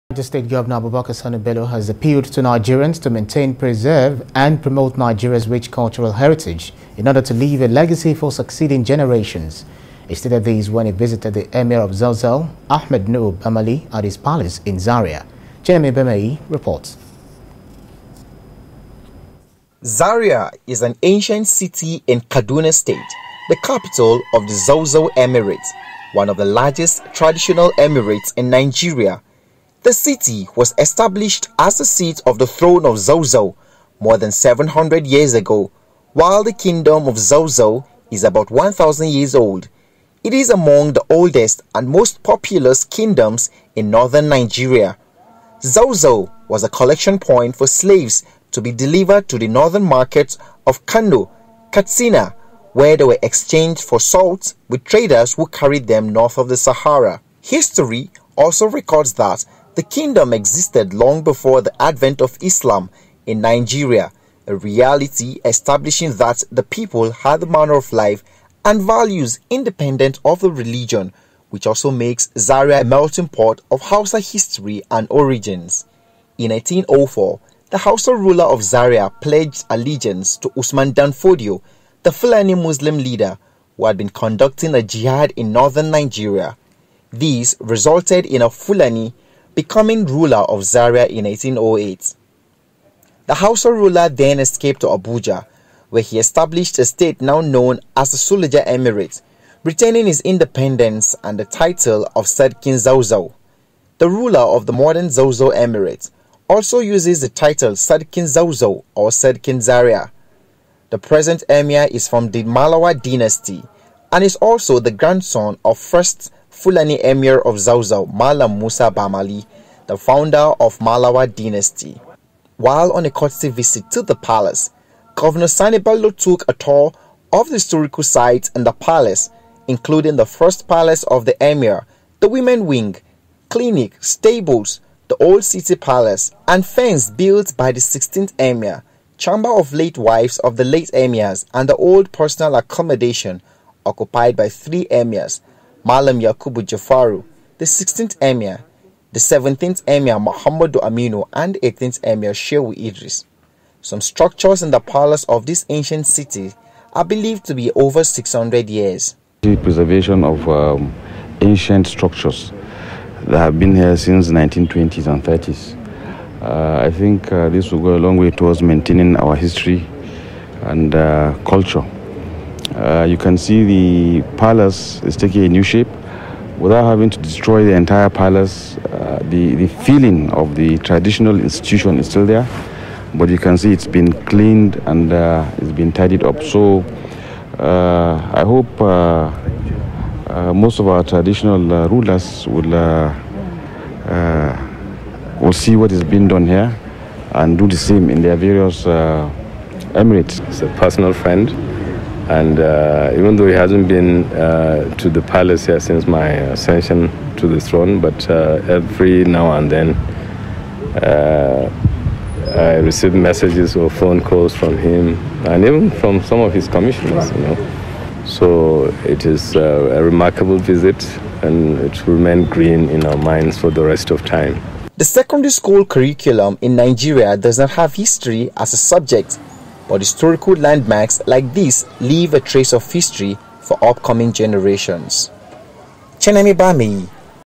[VIDEO] Governor Sani Bello campaigns for preservation of Heritage sites
Niger State Governor Abubakar Sani Bello has appealed to Nigerians to maintain, preserve and promote Nigeria's rich cultural heritage in order to leave a legacy for succeeding generations
He stated this when he visited the Emir of Zazzau, Ahmad Nuhu Bamali at his palace in Zaria.